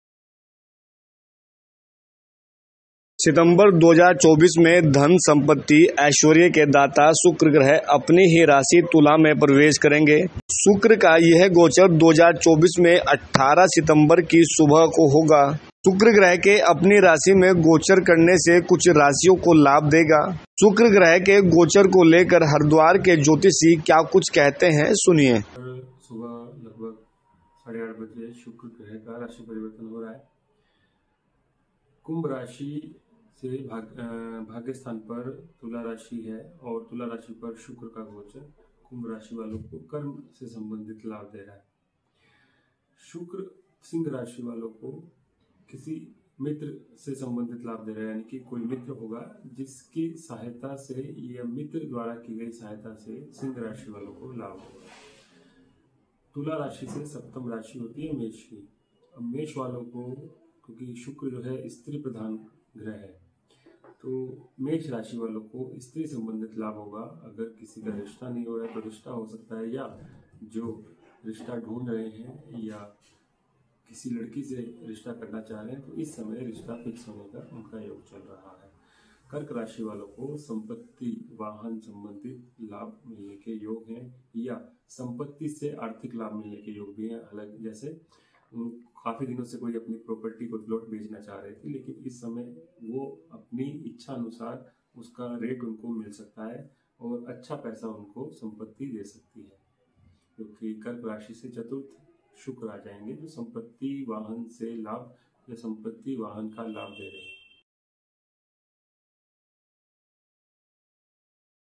Shukra Gochar 2024: शुक्र कर रहा है राशि परिवर्तन, इन राशियों को देगा शुभ फल | Vastu Tips
According to astrology, a person's birth chart includes a total of nine planets. All nine planets continuously transit from one zodiac sign to another over time. According to astrology, this transit of planets affects both the world at large and an individual's life, bringing both positive and negative influences. In September 2024, Venus, the planet associated with wealth, property, and luxury, will enter its own sign of Libra. This transit of Venus will occur on September 18, 2024.

ज्योतिष शास्त्र के अनुसार जातकों की कुंडली में कुल नौ ग्रह होते हैं. सभी नौ ग्रह एक निश्चित समय पर एक राशि से दूसरी राशि में परिवर्तन करते रहते हैं. ज्योतिष के अनुसार ग्रहों का यह राशि परिवर्तन देश दुनिया के साथ व्यक्ति के जीवन पर भी अपना अच्छा और बुरा प्रभाव डालते हैं. सितंबर 2024 में धन, संपत्ति, ऐश्वर्य के दाता शुक्र ग्रह अपनी ही राशि तुला में प्रवेश करेंगे. शुक्र का यह गोचर 2024 में 18 सितंबर को होगा.